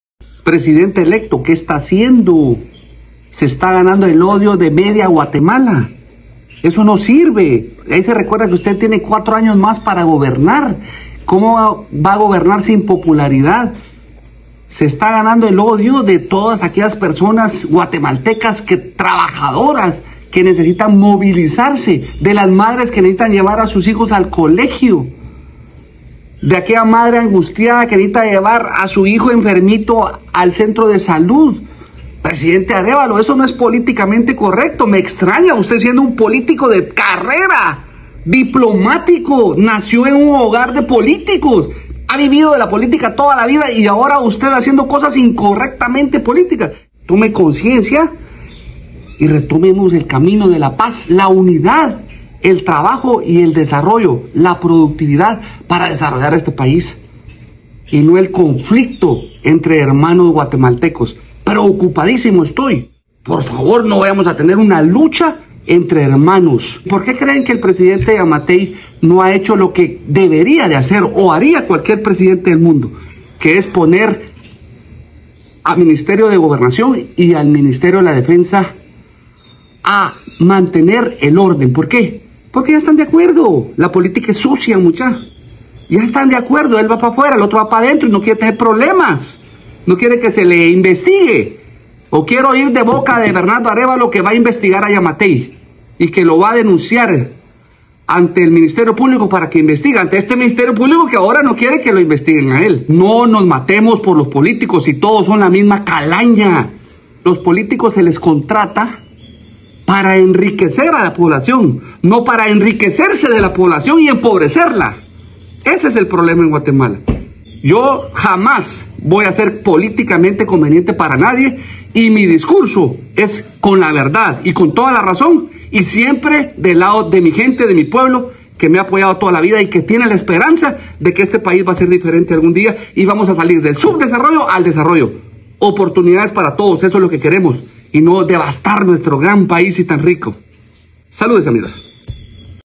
CUANDO EL CACIF SE VE AFECTADO SUCEDE ESTO:👇
CARLOS PINEDA ENVÍA UN FUERTE MENSAJE A BERNARDO ARÉVALO, "LAS PR0TESTAS AFECTAN A LOS GUATEMALTEC0S"

Según indica el expolitico carlos pineda pide que todo sea llevado por el camino de la paz.

Estos bloqueos están afectando el bolsillo de los guatemaltecos, o a todo aquel que quiere ir a trabajar.